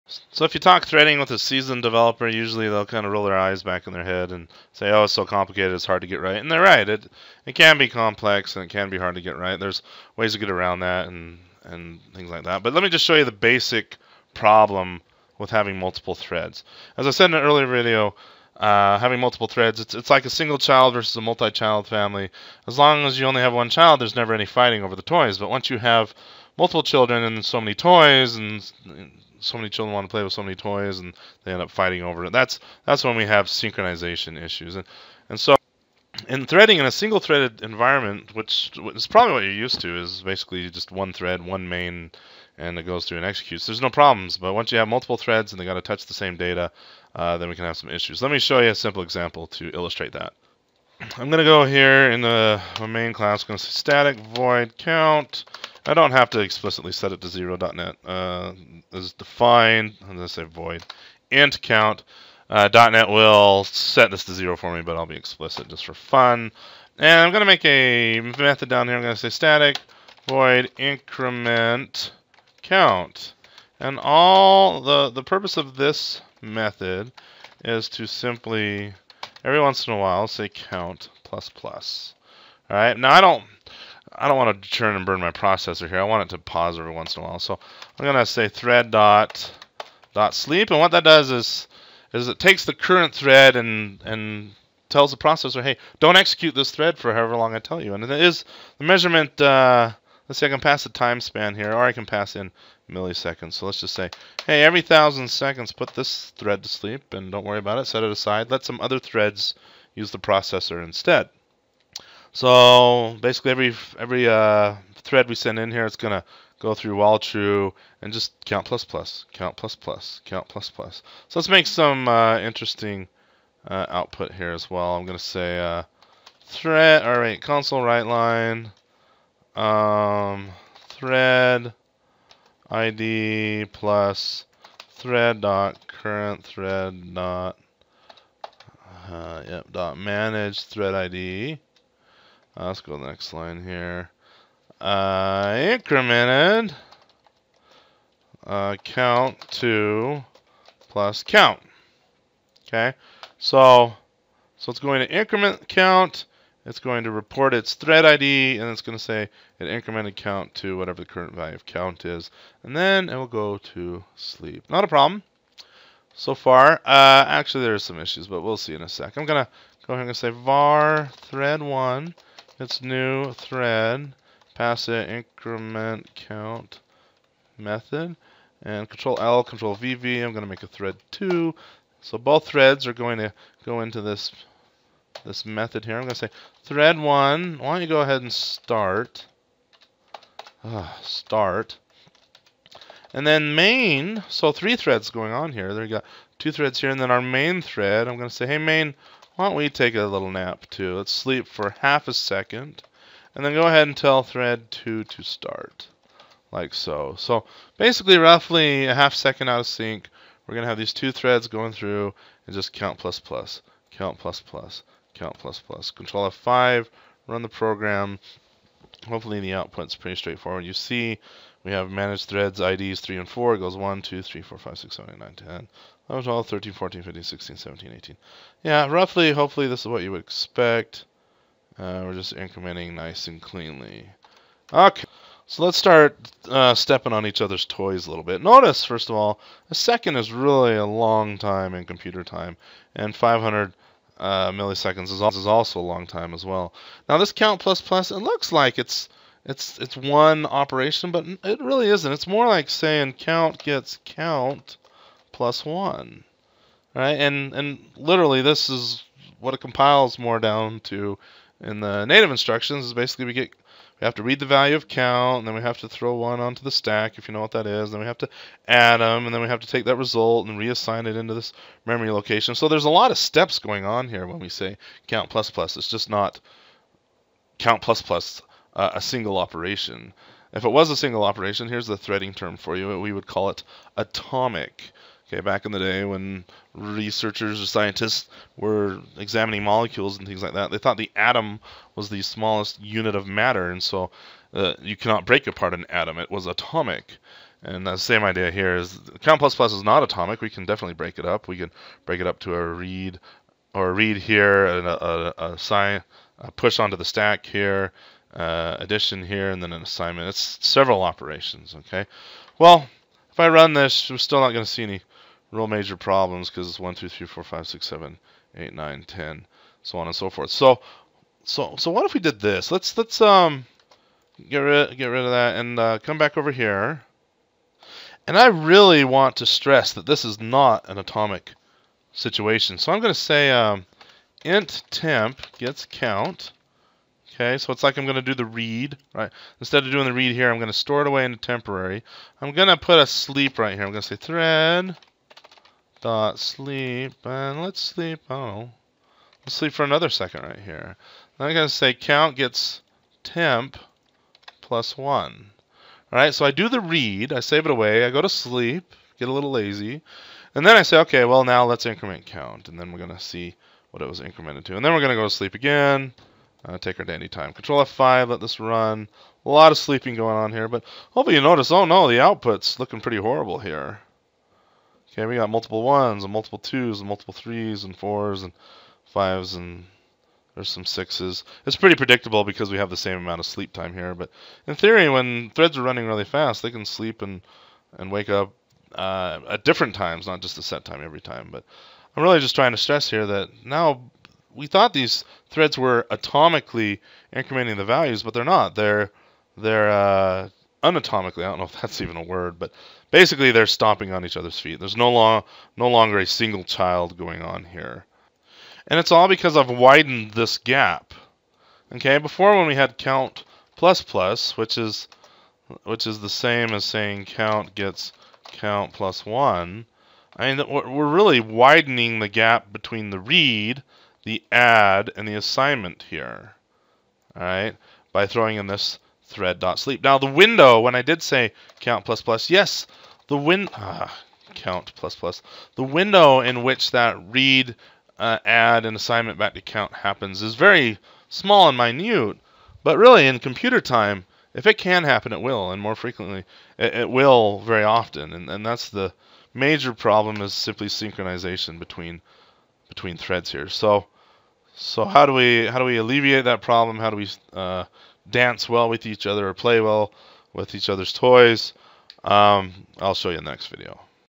C# Thread Synchronization Issue
Shows why thread synchronization is important in C# and some of the gotchas.